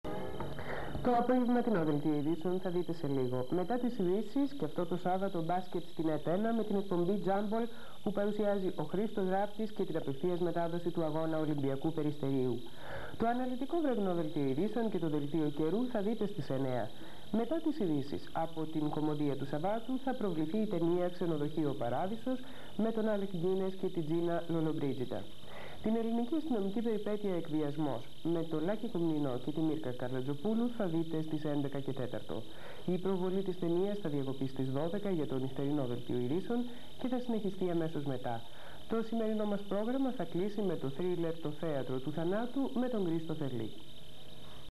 ΔΑΦΝΗ ΜΠΟΚΟΤΑ
Αναγγελία προγράμματος από την τηλεπαρουσιάστρια της ΕΤ-1 Δάφνη Μπόκοτα.